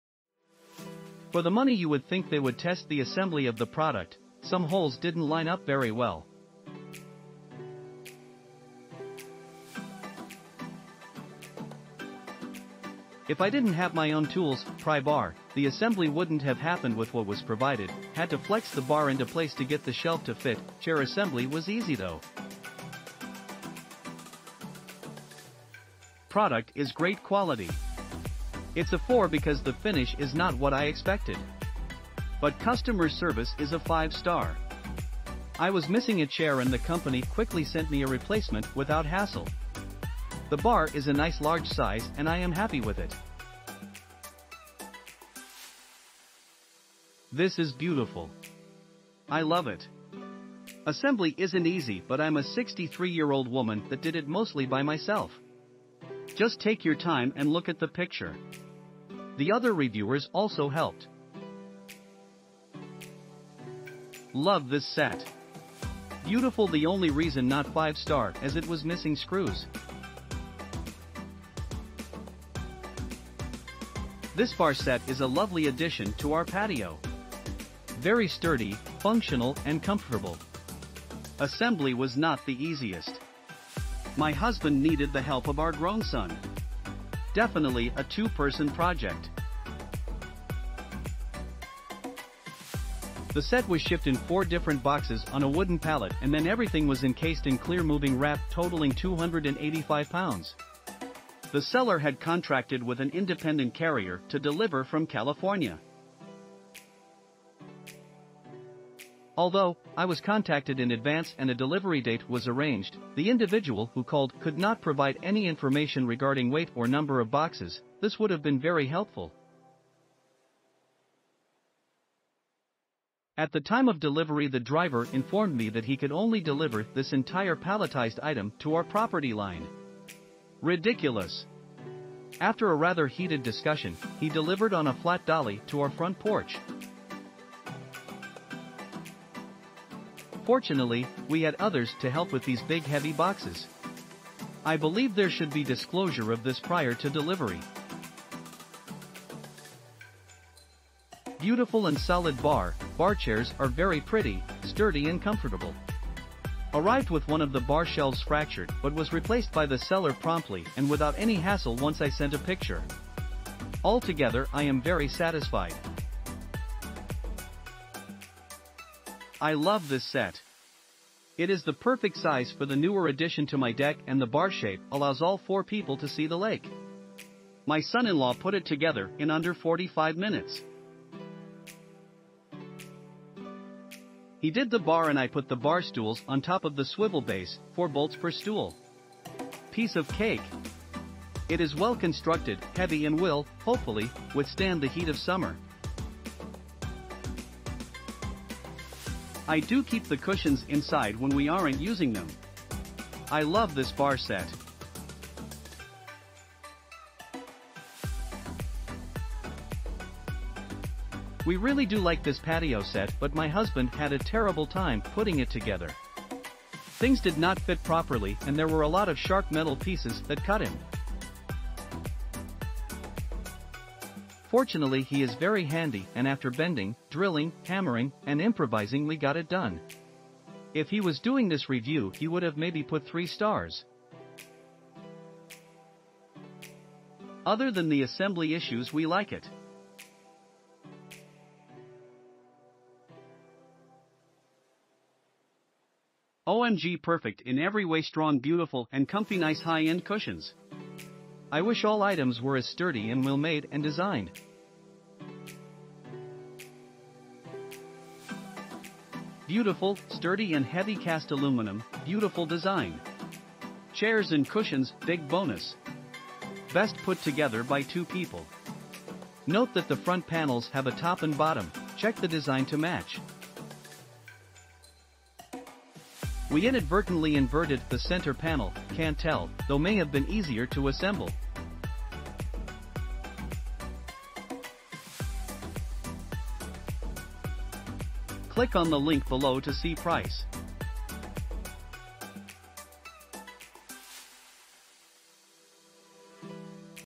Theworldofpatio Elizabeth Cast Aluminum Powder Coated 5pc Party Bar Set with Party Bar Table -...
- Theworldofpatio Elizabeth Cast Aluminum Powder Coated 5pc Party Bar Set with Party Bar Table - Antique Bronze

For The Money You Would Think They Would Test The Assembly Of The Product,Some Holes Didnt Line Up Very Well.
If I Didnt Have My Own Tools(Pry Bar) The Assembly Wouldnt Have Happened With What Was Provided,Had To Flex The Bar Into Place To Get The Shelf To Fit,Chair Assembly Was Easy Though.
Product is great quality. It's a four because the finish is not what I expected. But customer service is a 5 star. I was missing a chair and the company quickly sent me a replacement without hassle. The bar is a nice large size and I am...